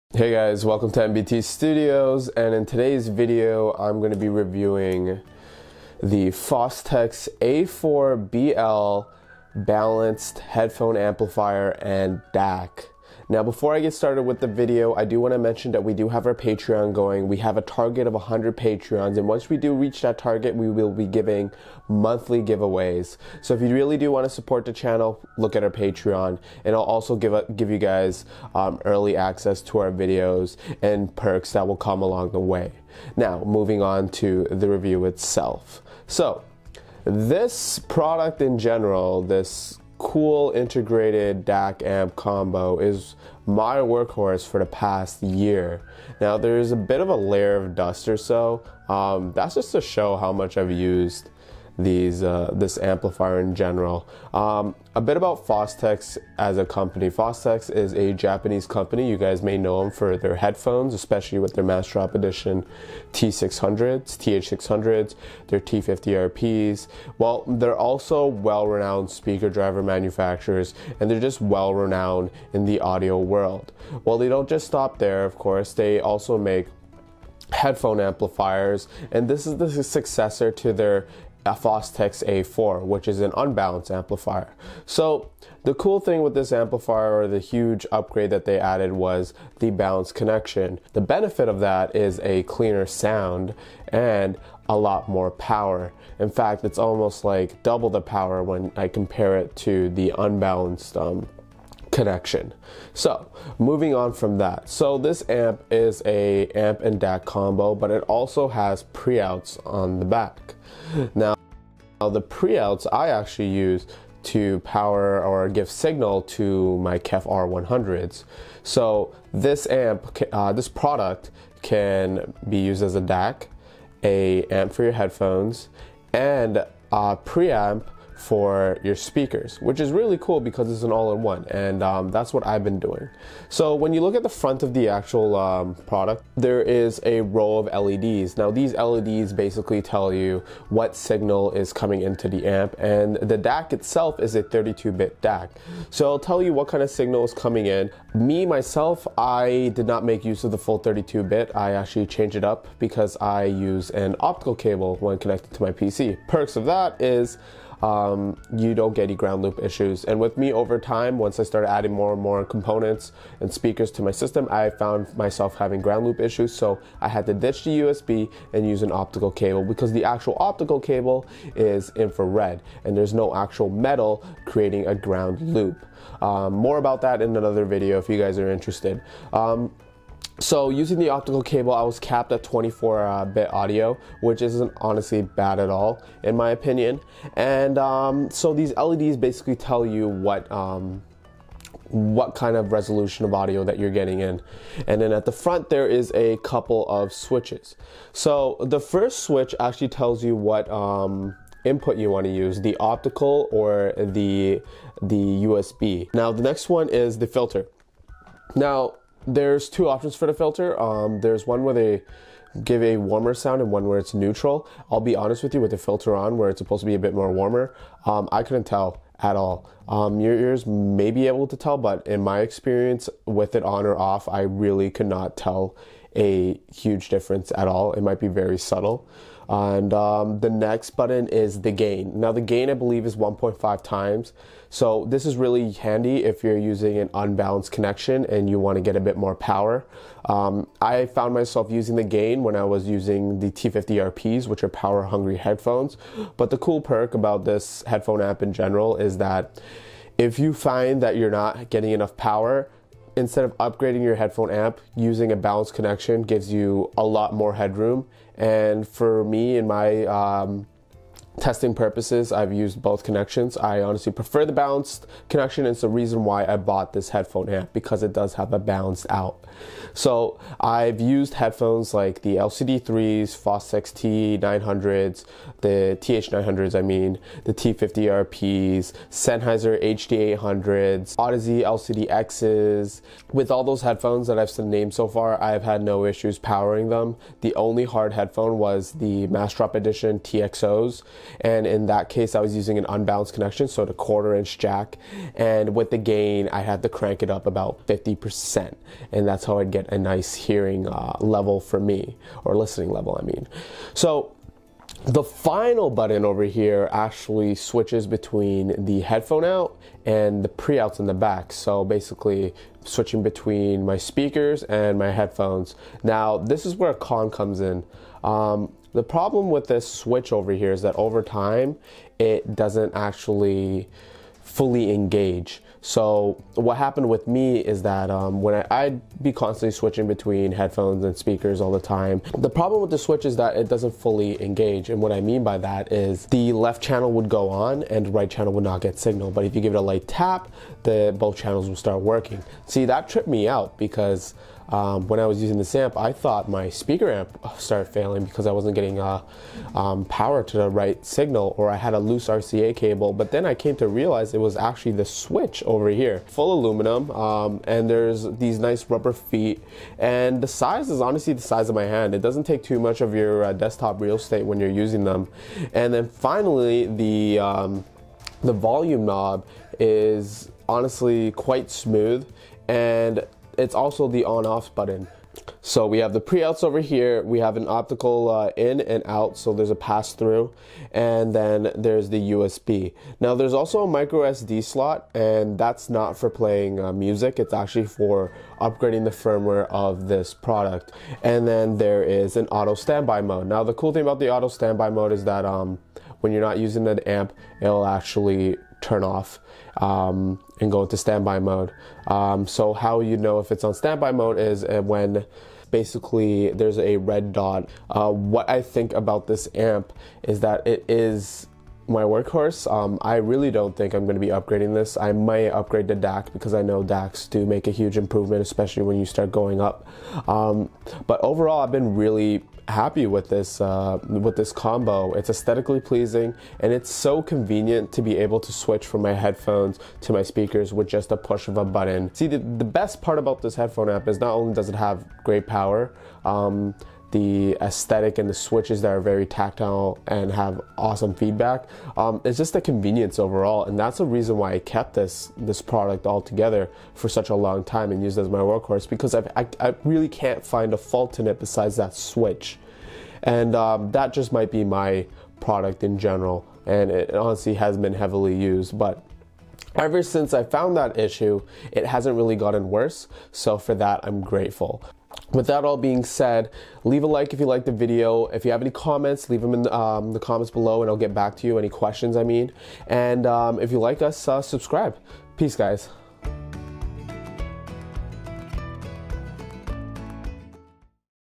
Fostex HP-A4BL amp/dac review - Still the best headphone amp?
Fostex a4bl is a great choice for a minimalist desktop set up.
Can be used as headphone amp, dac, and pre-amp for powered speakers.

fostex a4bl : (USA)
(UK) (only unbalanced version)

This video is made possible by supporters, if you would consider buying me a cup of coffee because you know I LUVVV coffee